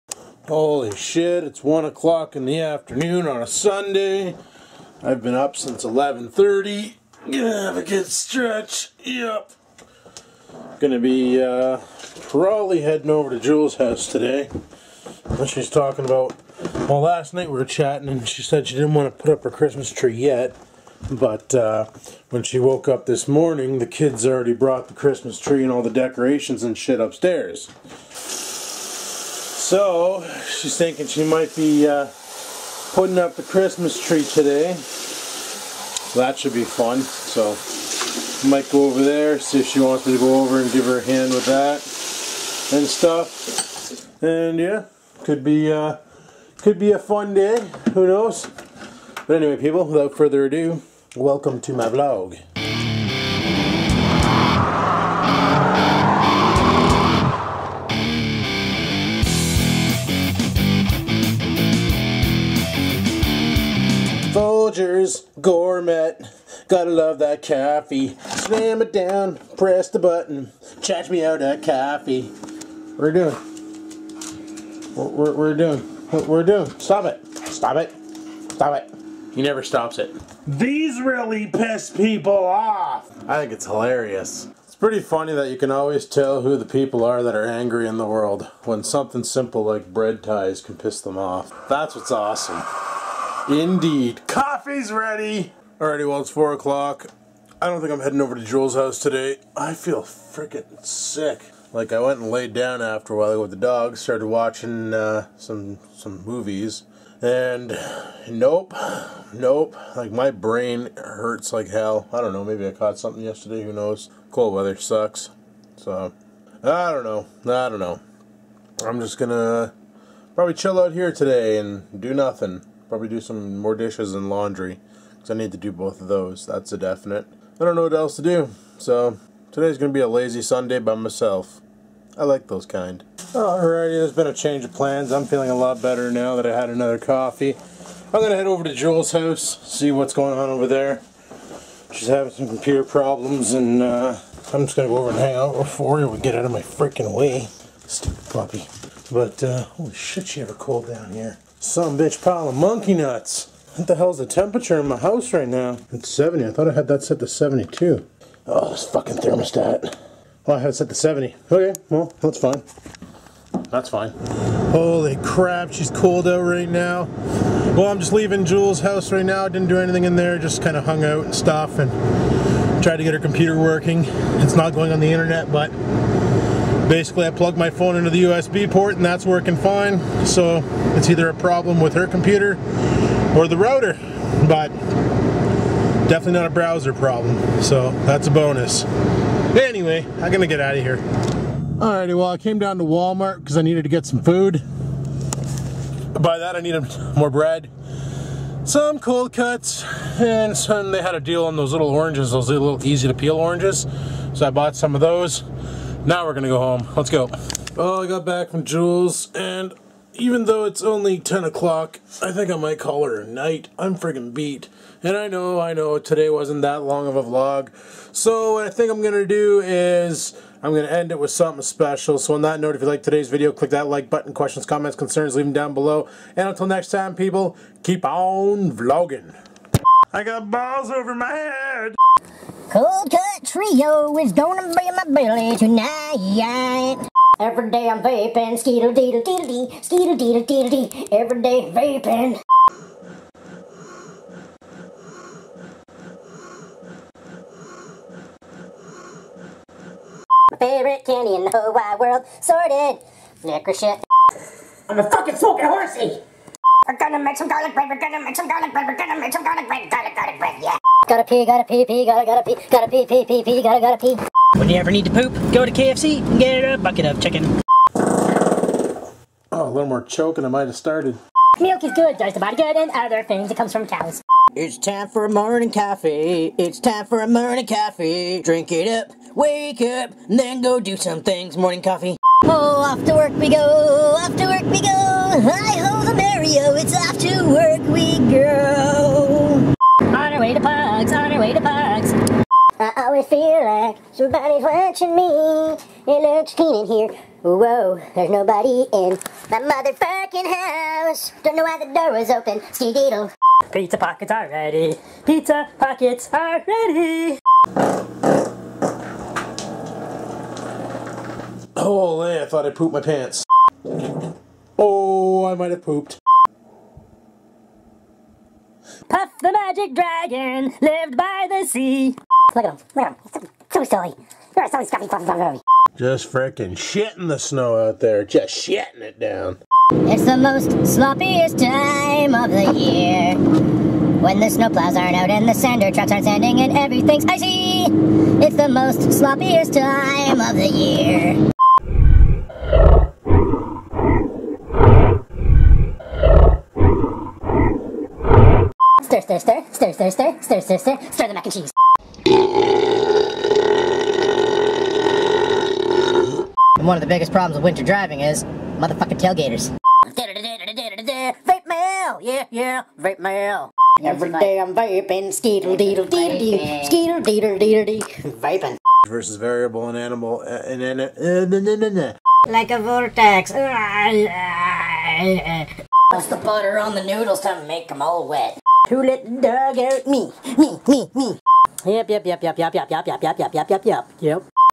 Super Lazy Sunday with WTF Moments
Well I am feeling a little under the weather today so I decided to add a little something special at the end of the vlog.  enjoy.